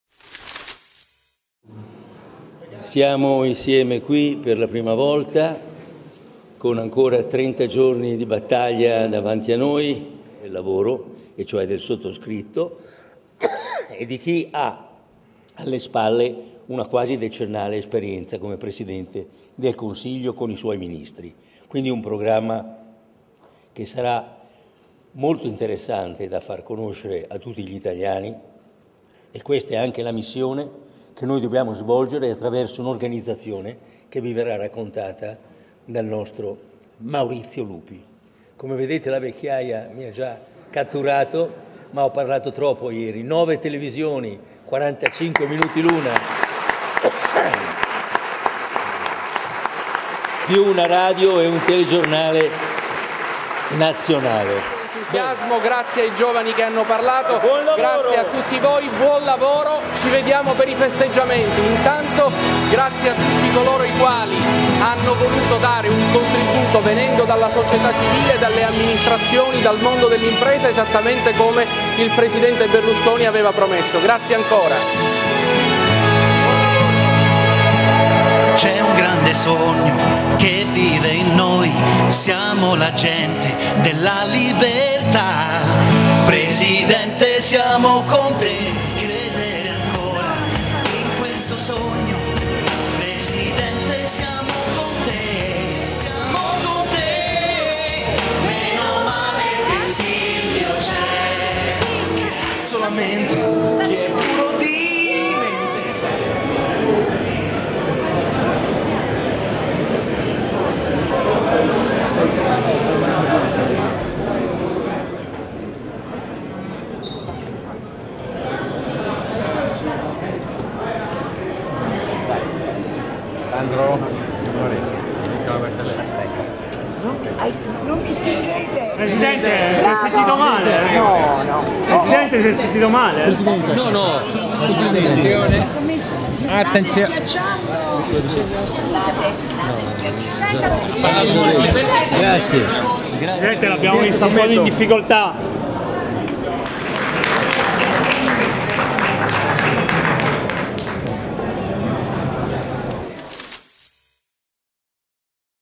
Pdl, malore per Silvio Berlusconi al Capranica di Roma
Terminato il primo incontro con i candidati del Popolo della Libertà, Silvio Berlusconi, dopo due ore di monologo dal palco, il presidente Berlusconi ha un mancamento e dopo alcuni minuti in una sala del teatro Capranica al fattoquotidiano nega di essersi sentito male.